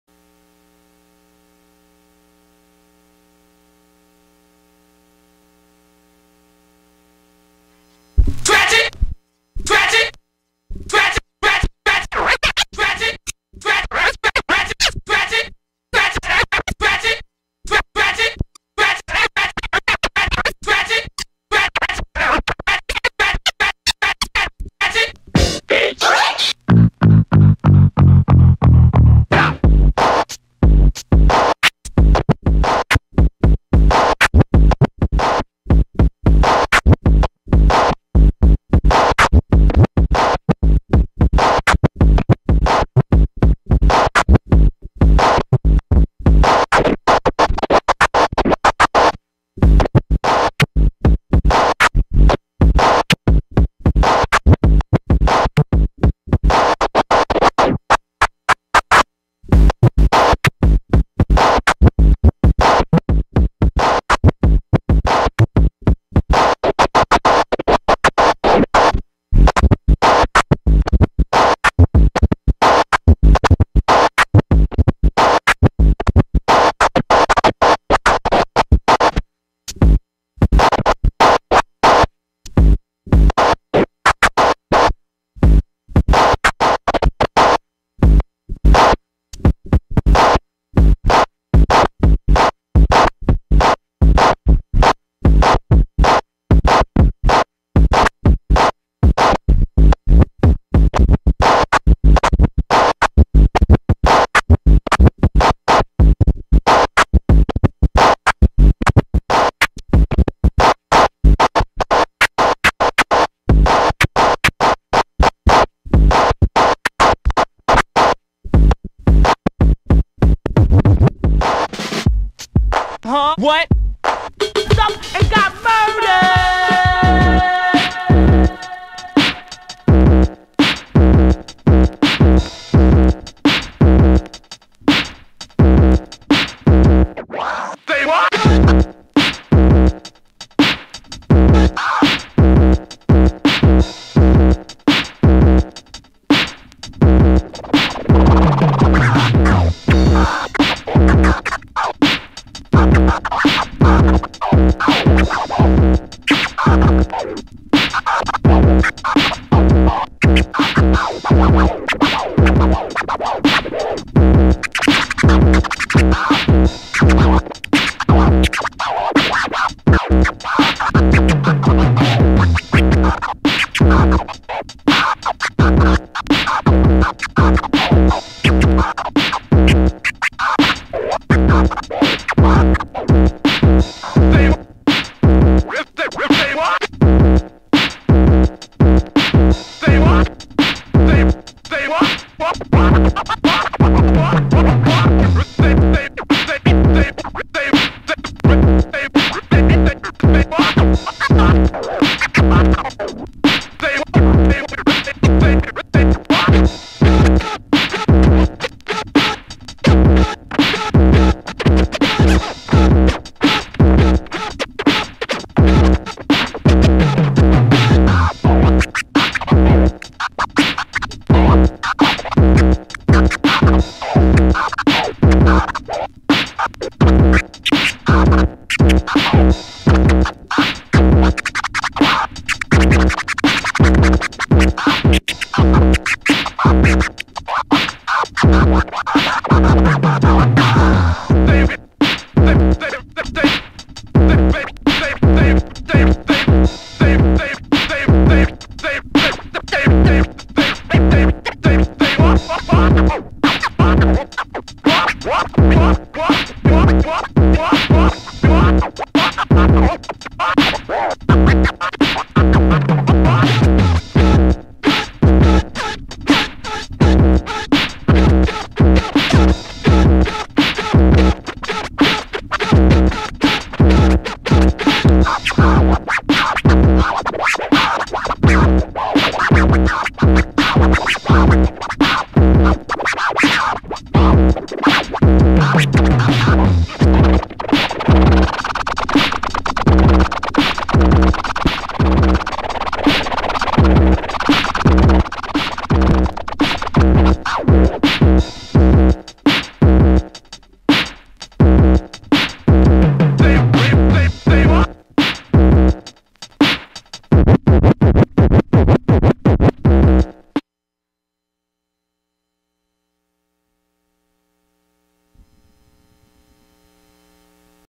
DJ Deez New Mixer Garbage Scratch... Welcome to the Gun Show.
First time on my "new" used Rane mixer.